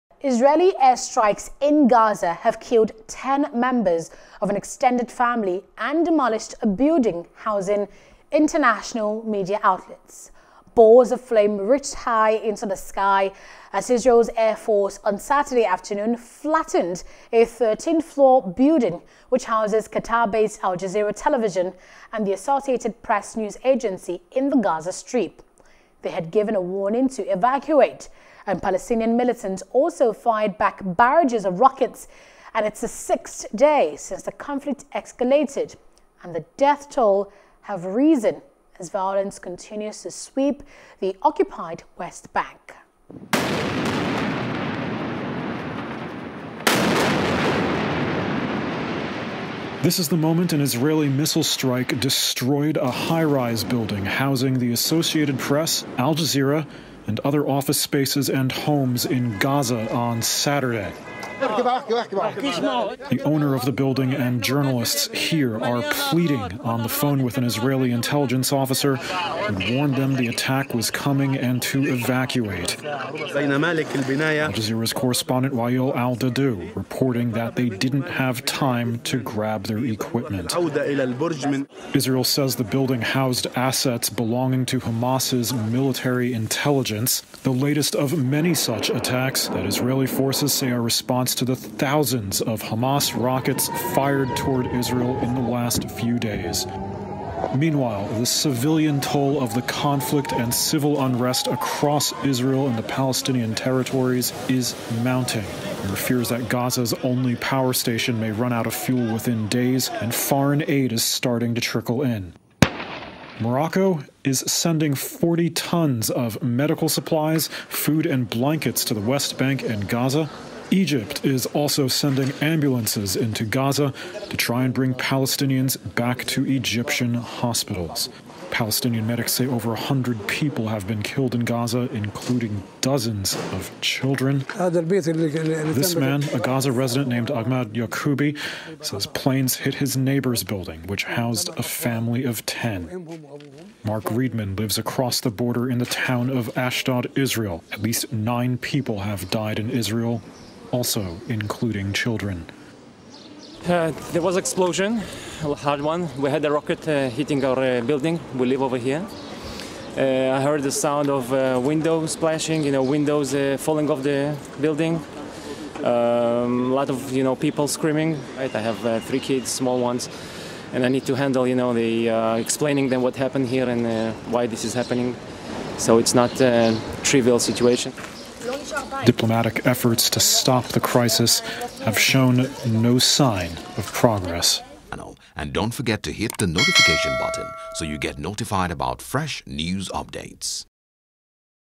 Israel Pounds Gaza, Hits Media Building Housing AP And Al-Jazeera | FOREIGN
Israeli air strikes in Gaza have killed 10 members of an extended family and demolished a building housing international media outlets.